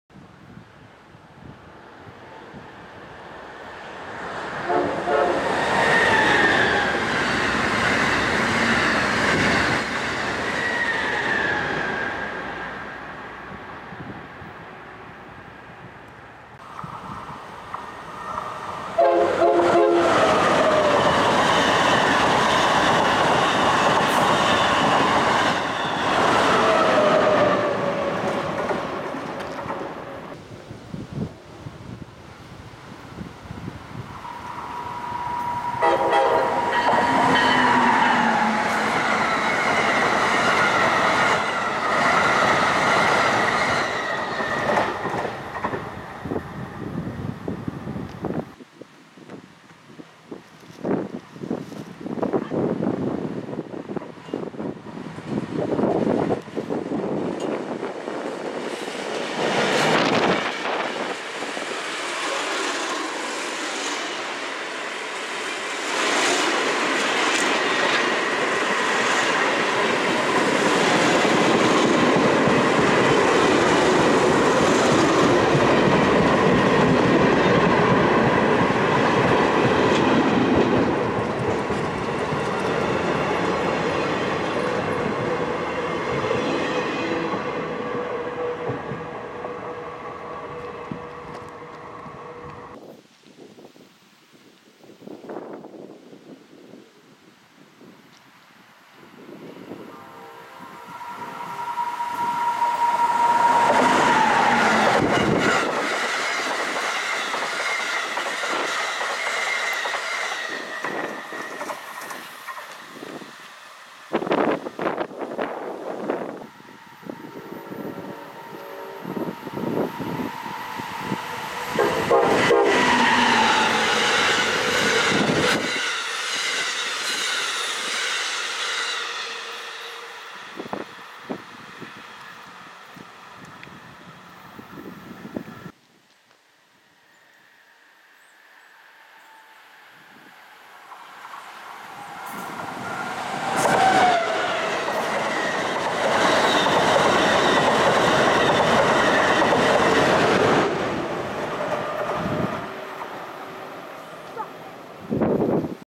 Amtrak Train Action Near Burrows Field Park In Groton, CT (9/?? & 10/1/20)
Amtrak train action near Burrows Field Park in Groton, CT including an undercutter train.